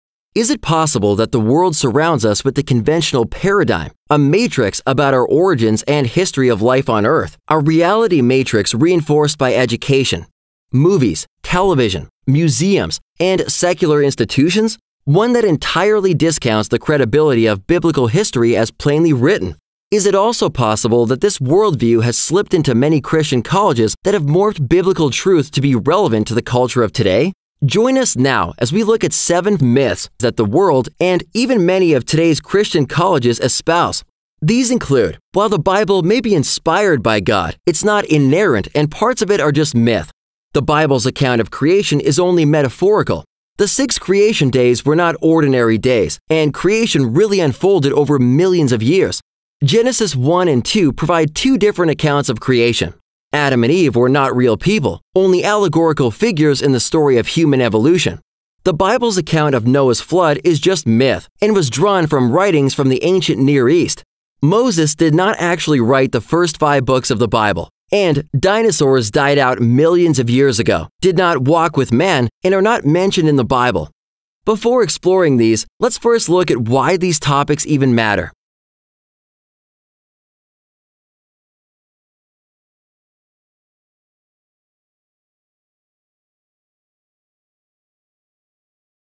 Debunking the Seven Myths about Genesis, Creation, and the Flood (Overview)
Overview of our forthcoming "Debunking the 7 Myths that Deny Biblical Truth" video series (to be released by October 2019). The 7 myths include:

1 - “While the Bible may be ‘inspired,’ by God, it’s not ‘inerrant’ and parts of it are just myth.”
2 - “The Bible’s account of Creation is only metaphorical, the six creation days were not ordinary days, and creation really unfolded over millions of years.”
3 - “Genesis 1 and 2 provide two different accounts of creation.”
4 - “Adam and Eve were not real people, only allegorical figures in the story of human evolution.”
5 - “The Bible’s account of Noah’s Flood is just myth and was drawn from writings from the Ancient Near East.”
6 - “Moses did not actually write the first five books of the Bible,” and
7- Dinosaurs died out millions of years ago, did not walk with man, and are not mentioned in the Bible.”

Donations to our "Debunking the 7 Myths" project can be made